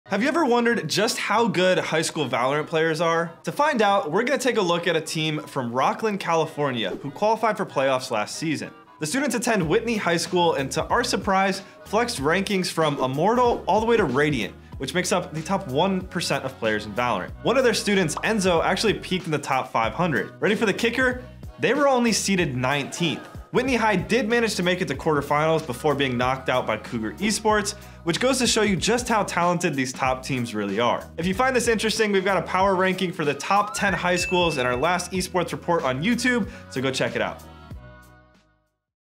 High School Esports Report: Whitney High Valorant
This High School Esports team was made up of cracked students.

A Valorant team from ____ placed _____ in the HSEL Fall Major.

Despite having a team of Immortals and Radiants, they still couldn't clench a spot to finals.

Competitive gaming has only just begun so it'll be fun to watch all of these students grow and become more talented!
Written & Produced by: Gracie Star (@graciesta.r)
Hosted by: Coach Alne (@coachalne)
Filmed & Edited by: Alex Acosta (@alexekko_)